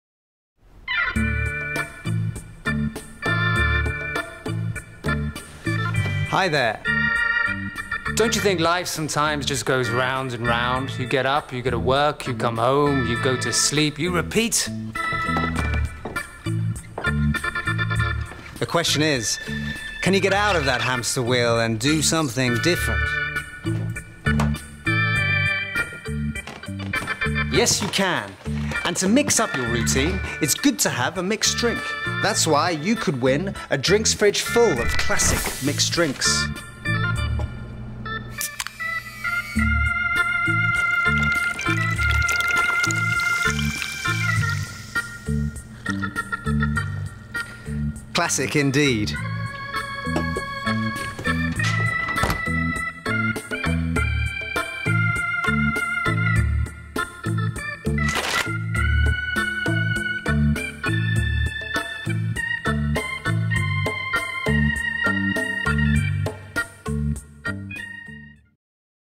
Classic mixed drinks : Hamster Wheel. Yes you can.
Can you drink a Gordon's Gin and Tonic or Smirnoff and Cola while playing the bongos at home? Yes you can.